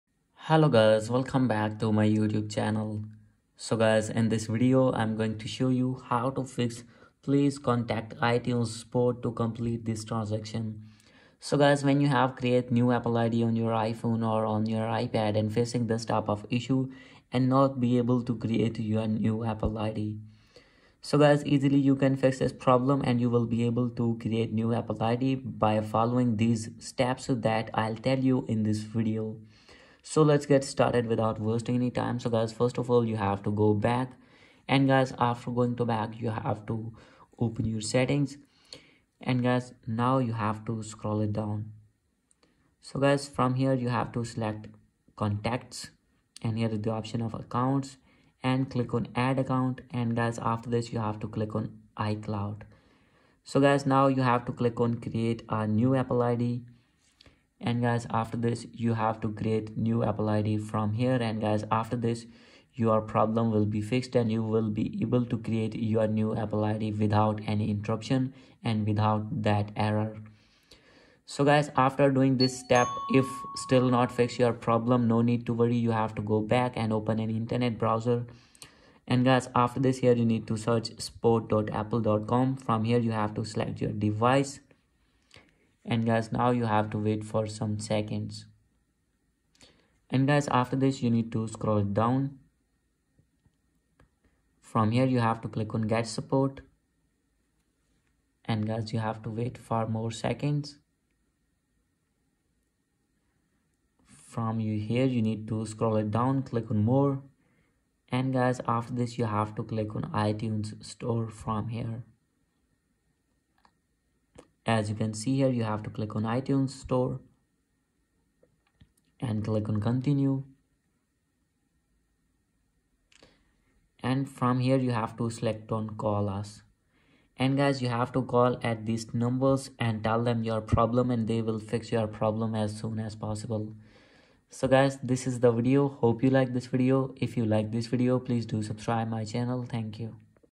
[iPhone]✅: Please Contact iTunes Support to Complete This Transaction When Creating Account [2024]
In this video, I will help you to fix please contact itunes support to complete this transaction when creating account. Please watch till the end.